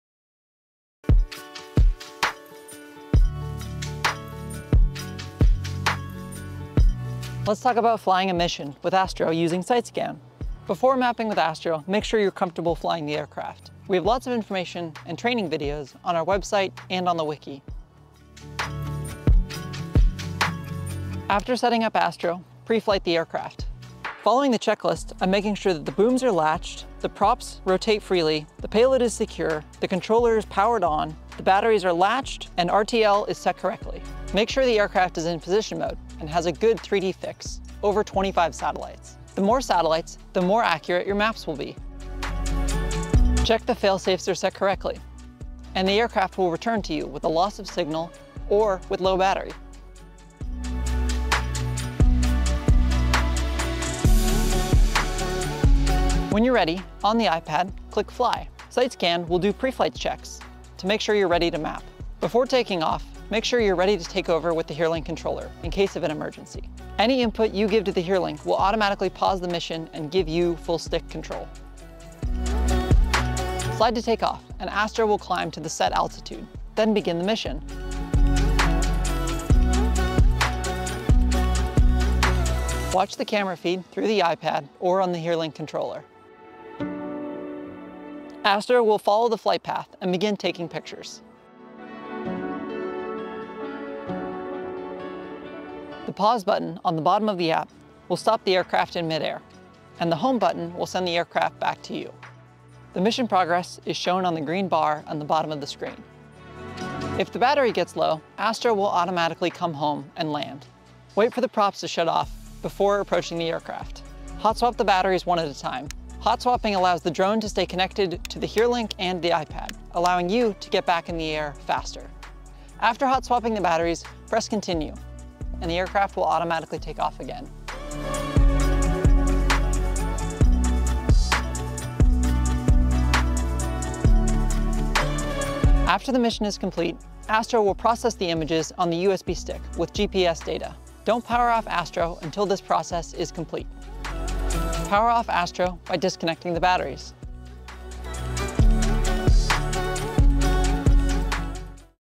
Flight | Astro + Site Scan Tutorial
This is part three of a series of tutorials to help you get up and running with Astro + Site Scan.

Freefly Astro, integrated with Site Scan for ArcGIS provides a complete end-to-end drone mapping solution. From drone hardware, automated flight planning and execution, online and offline, to imagery processing on the cloud or on desktop, to advanced analysis in ArcGIS…

Sync ID: MB01OAKWTUMG2JL